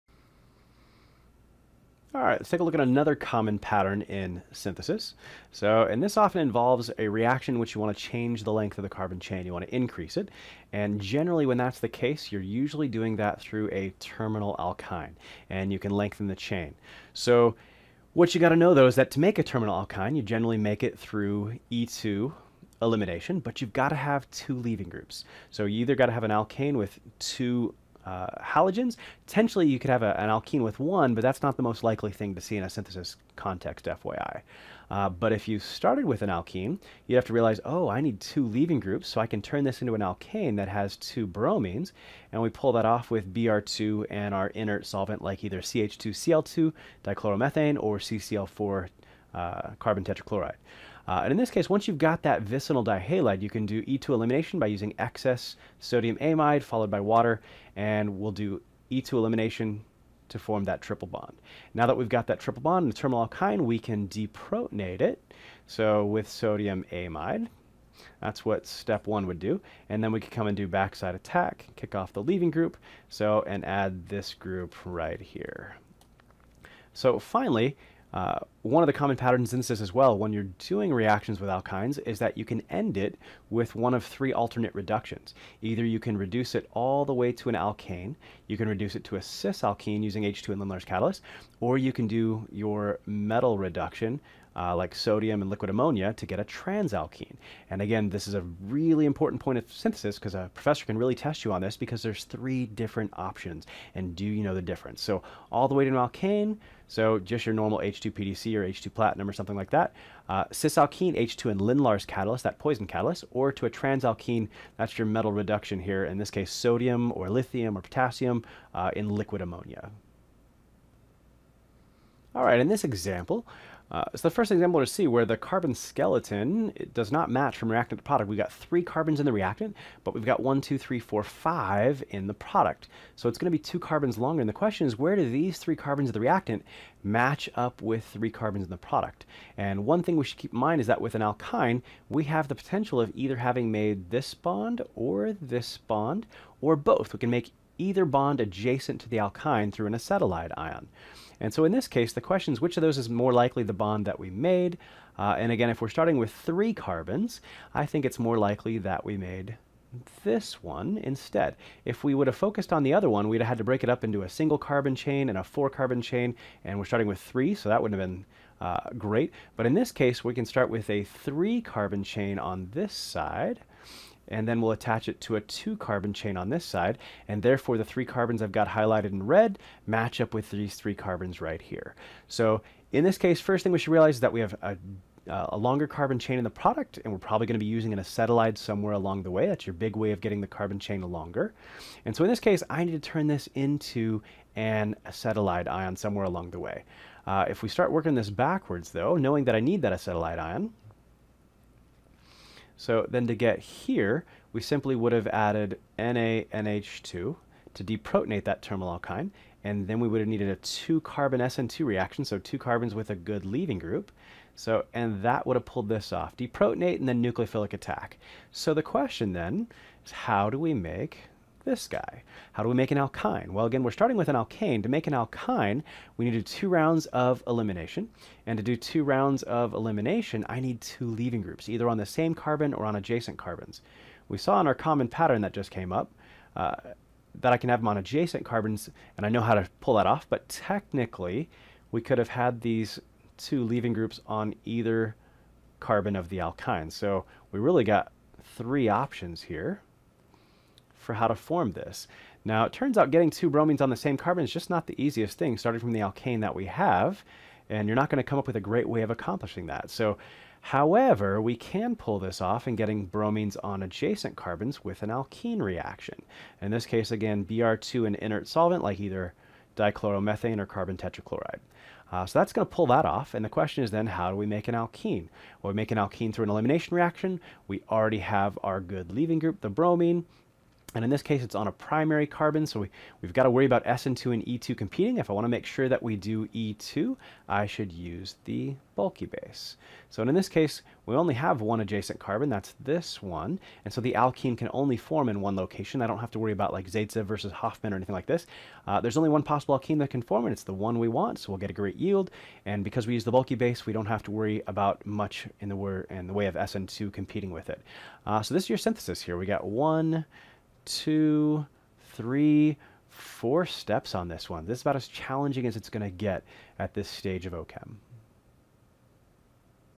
11.4c Common Patterns in Synthesis Part 3
Chad breaks down a common pattern in Synthesis: converting an Alkene to an Alkyne via a vicinal dihalide followed by Acetylation of the Alkyne.

Happy Studying!